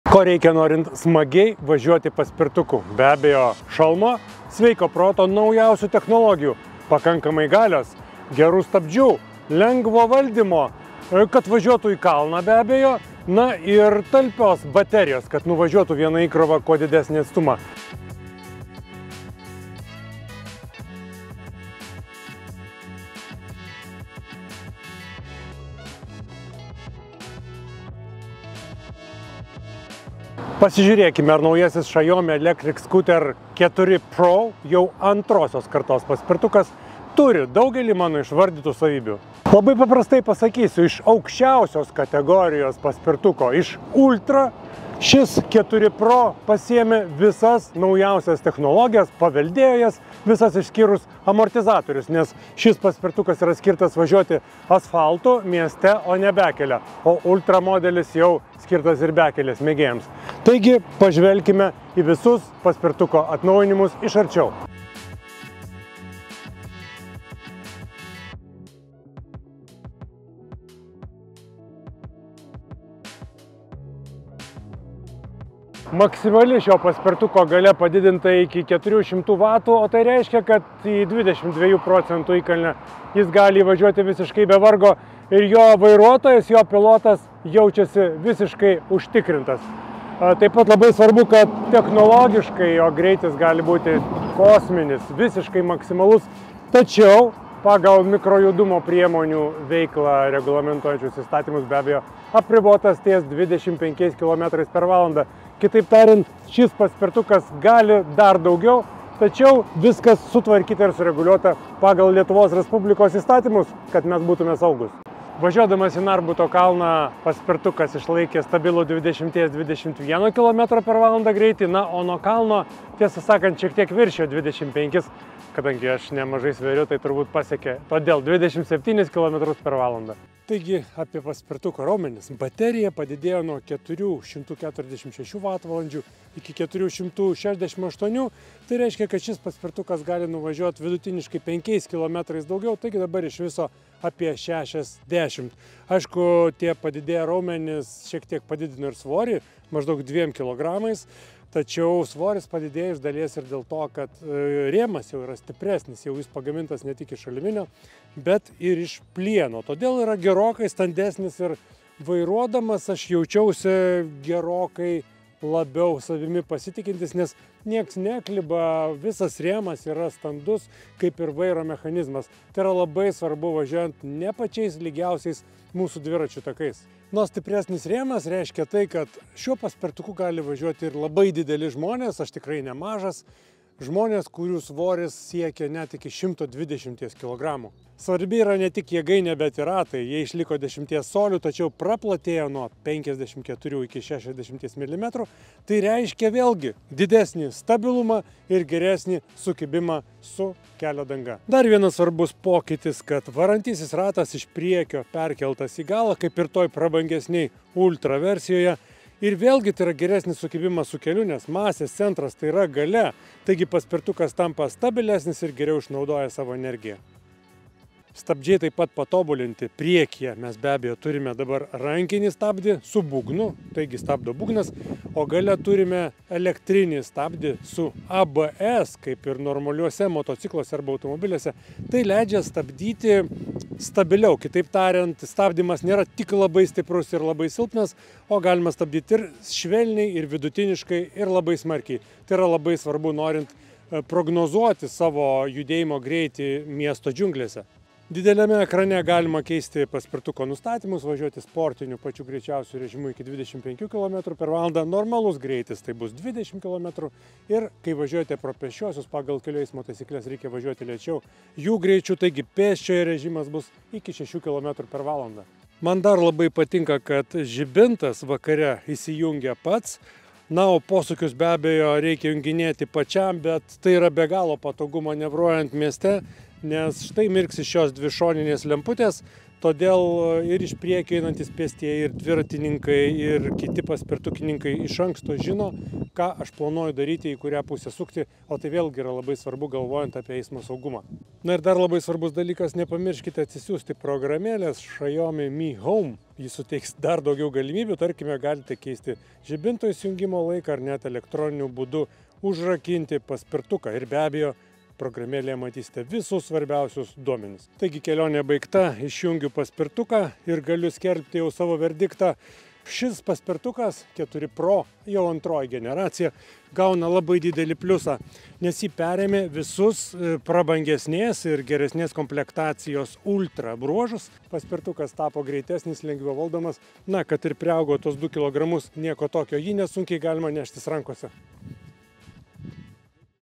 „Gazas“ išbando – jūs važiuojat: ką žada antros kartos Xiaomi Electric Scooter 4 Pro paspirtukai?
Ko reikia norint smagiai važiuoti paspirtuku? Žinoma, šalmo, įgūdžių, sveiko proto ir gero paspirtuko. O geras paspirtukas – tas kuris toli nuvažiuoja ir negriūva ant šono. Pasižiūrėkime, kokias funkcijas siūlo naujieji antros kartos Xiaomi Electric Scooter 4 Pro paspirtukai?

Reklama: ACC Distribution.